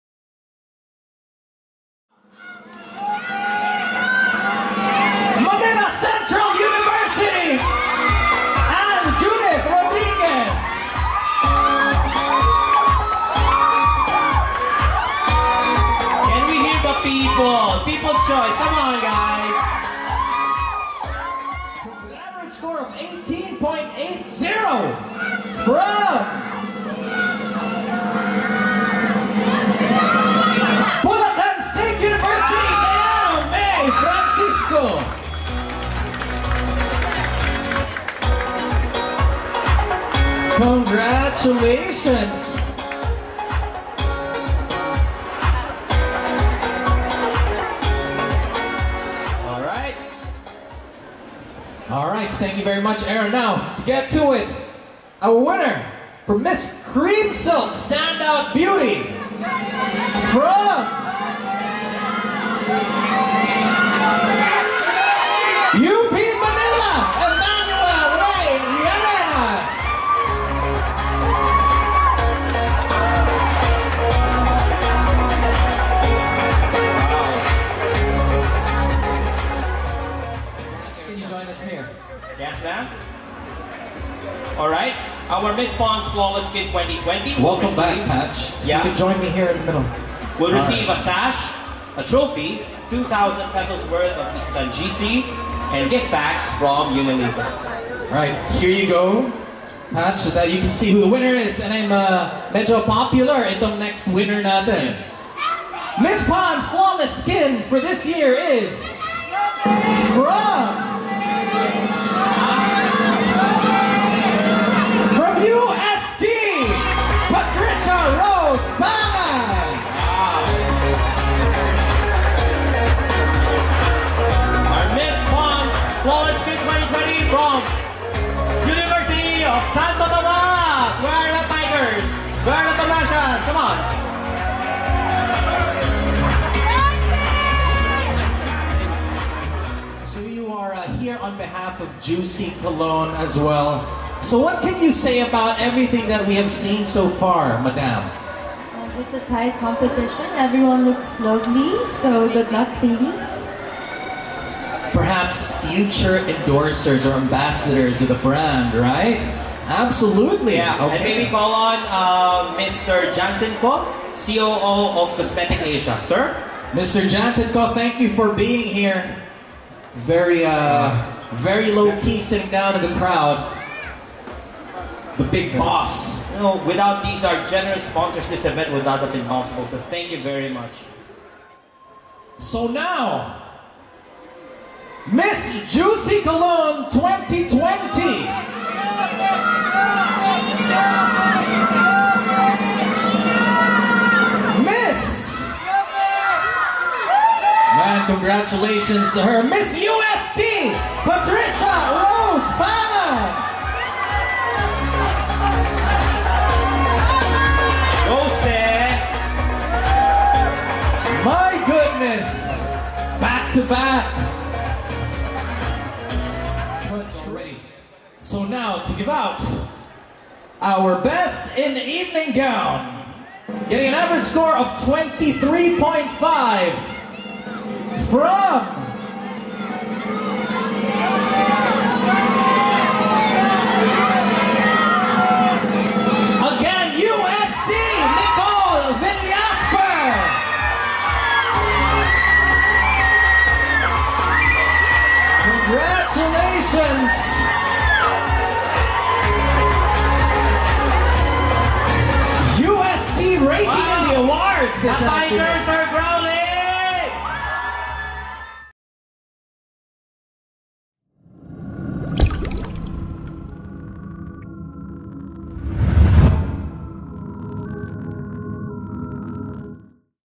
OFFICIAL: MISS U-BELT 2020, Special Awards.
An annual intercollegiate beauty pageant participated by students from various colleges & universities in the Philippines. We do not claim ownership of all music materials used in the actual event and in the video.
This event is presented by, Isetann Dept., Store, Inc., Pond's, Cream Silk, Juicy Cologne.